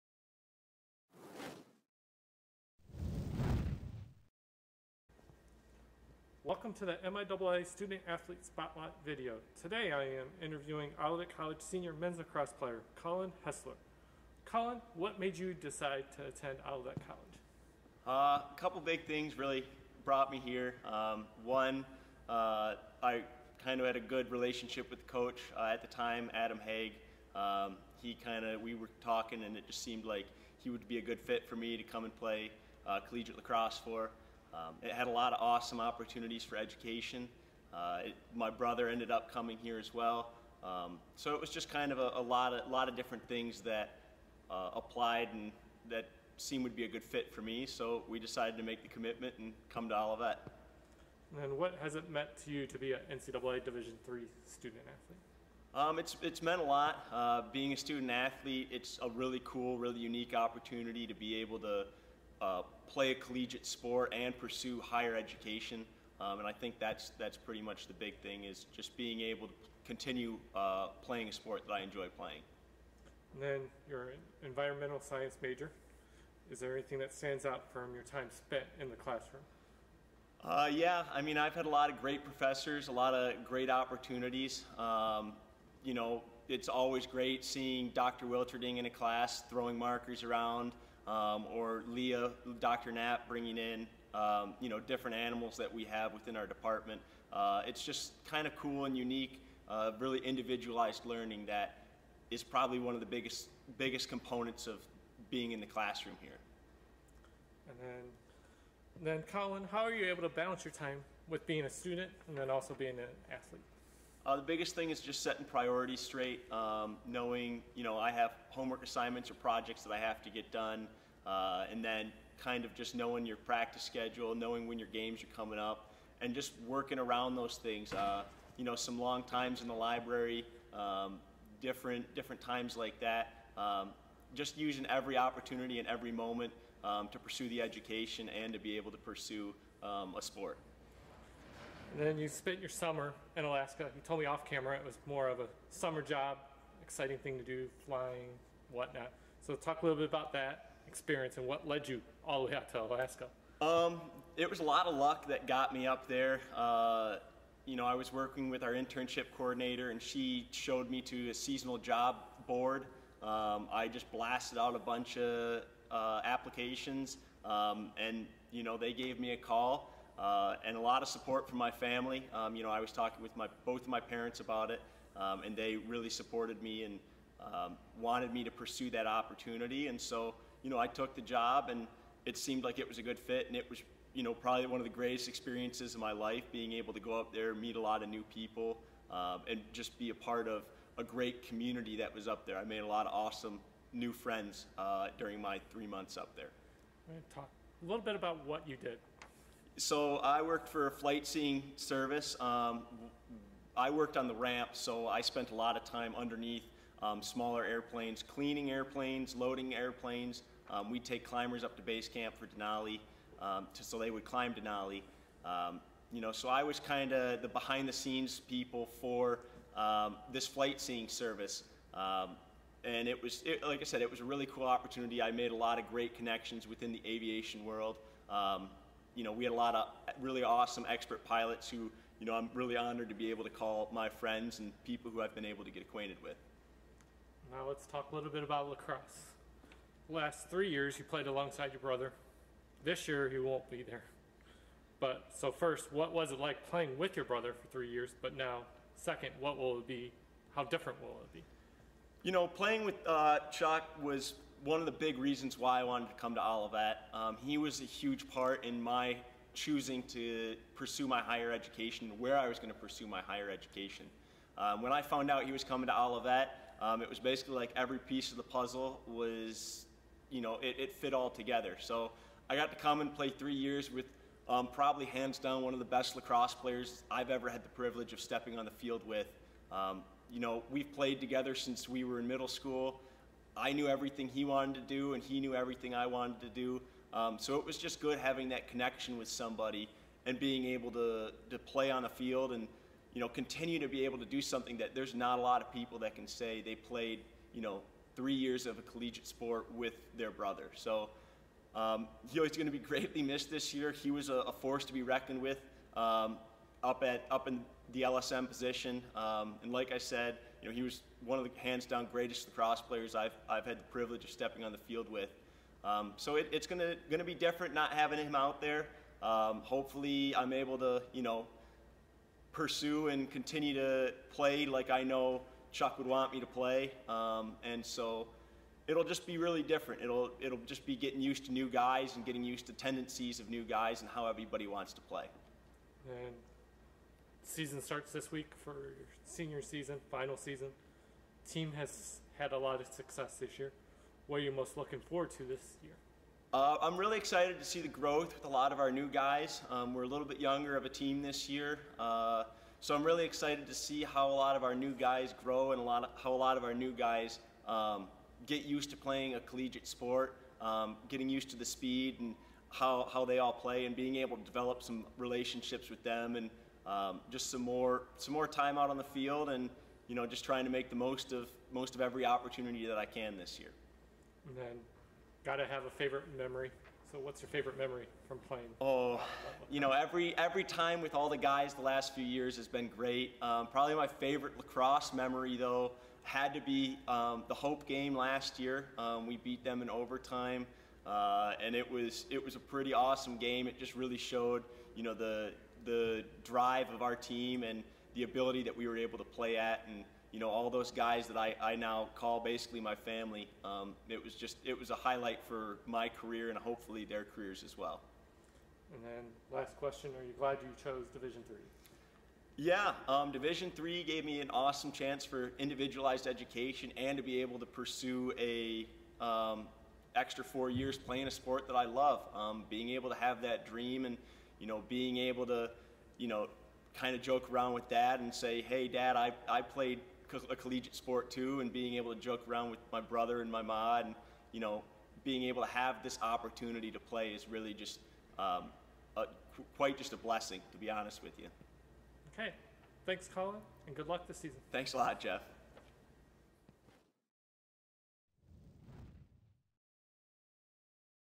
D3MIAA Student-Athlete Spotlight: Cullen Haesler, Olivet College
Olivet College's Cullen Haesler discusses various topics as part of the MIAA's Student-Athlete Spotlight video series.